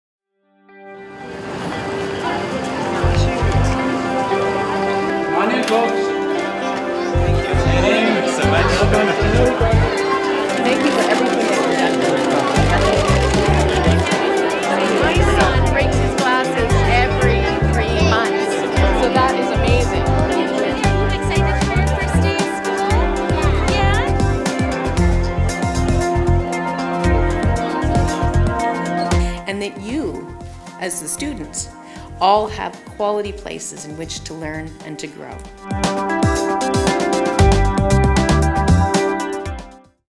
Premier and Education Minister visit new state-of-the-art Nellie Carlson School in Edmonton
Premier Rachel Notley and Education Minister David Eggen are on hand for the opening of the new Nellie Carlson School in south Edmonton, one of 32 projects to open in September.